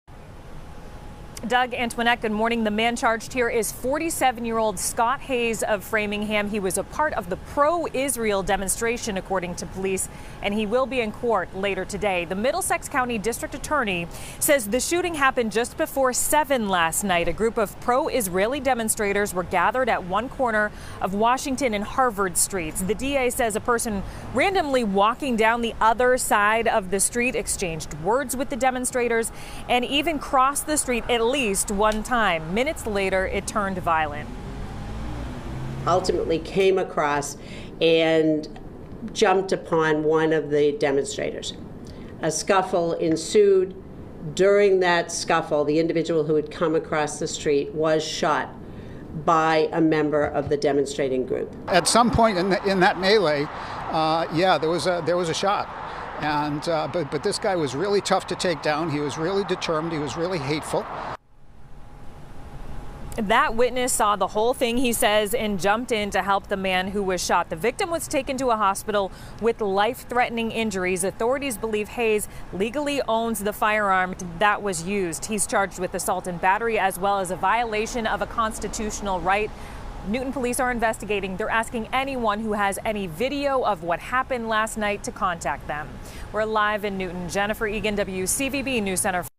Mn shot during fight at pro-Israel rally
Charges have been filed against a Framingham man after a confrontation and shooting at a pro-Israeli rally in Newton, Massachusetts, on Thursday evening.